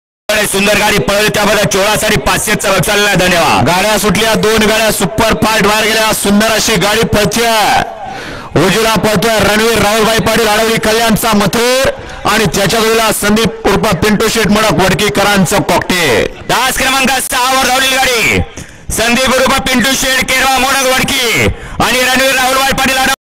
३/१/२०२४ वडकी मैदानातला मथुर 1001 & सुंदर (कॉकटेल) गट पास गाडी सेमी साठी पात्र🔥🔥👍🏻*
*🤩३/१/२०२४ श्री दत्त जयंती उत्सवानिमित्त भव्य दिव्य बैलगाडा शर्यत वडकी मैदानातला गट क्रं :- 4 तास क्रं :- 6 वर धावलेली गाडी मथुर 1001 & सुंदर (कॉकटेल) गट पास गाडी सेमी साठी पात्र🔥🔥👍🏻*